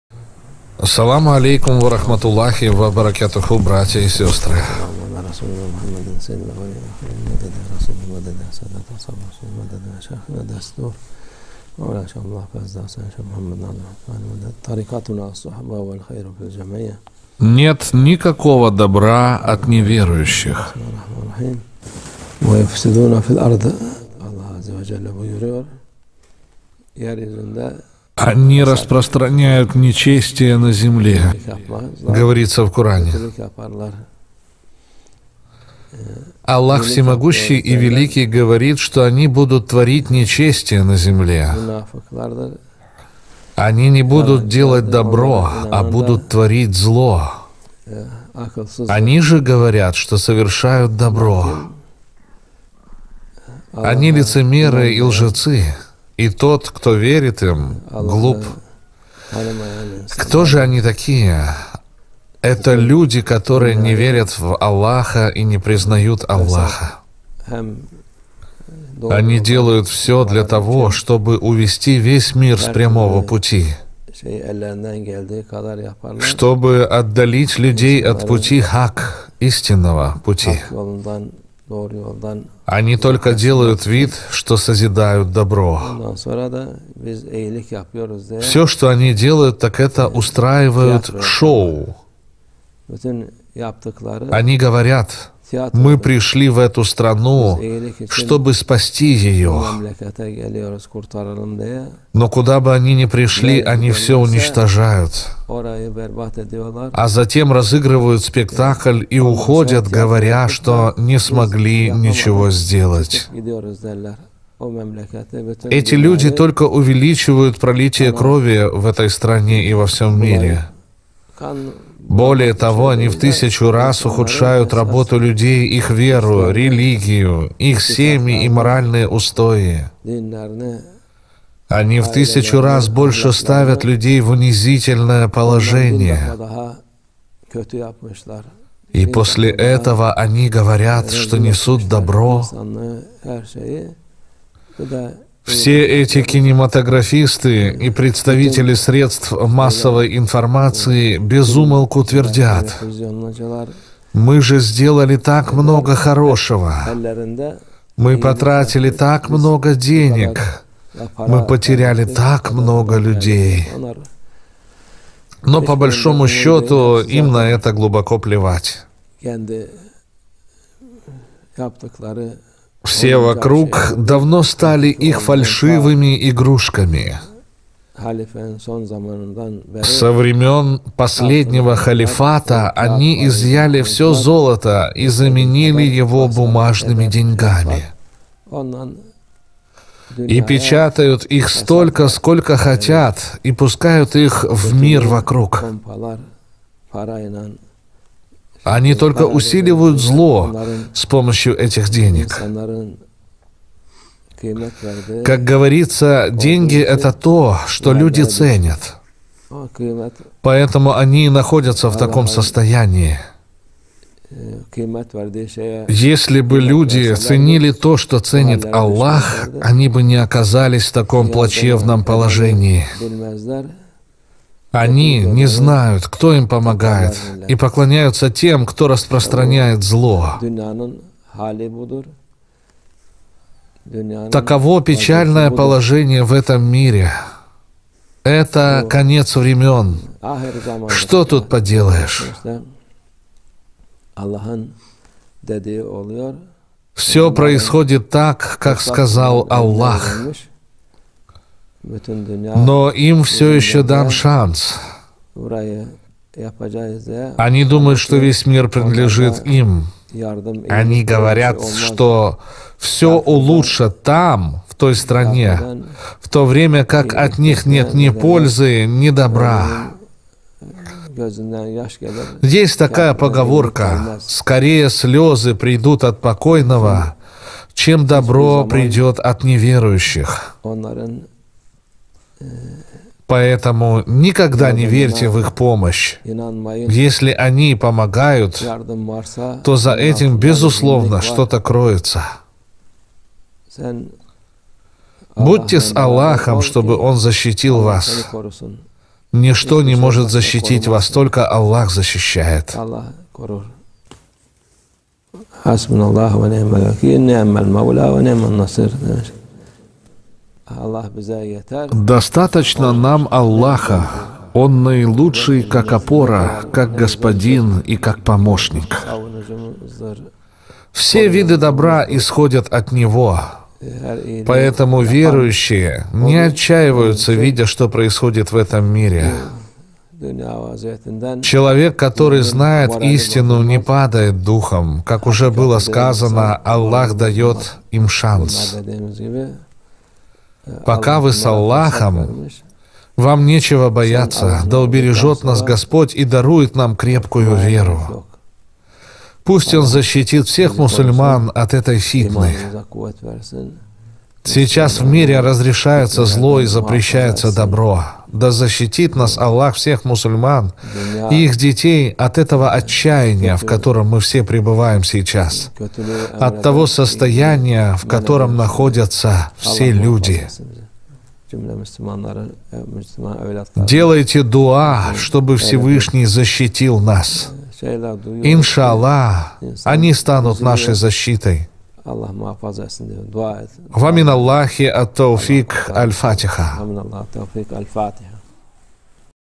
Шейх Мехмет Эфенди. Нет никакого добра от неверующих
Они "распространяют нечестие на земле" (Коран 02:27).
Аллах Всемогущий и Великий говорит, что они будут творить нечестие на земле. Они не будут делать добро, а будут творить зло. Они же говорят, что совершают добро.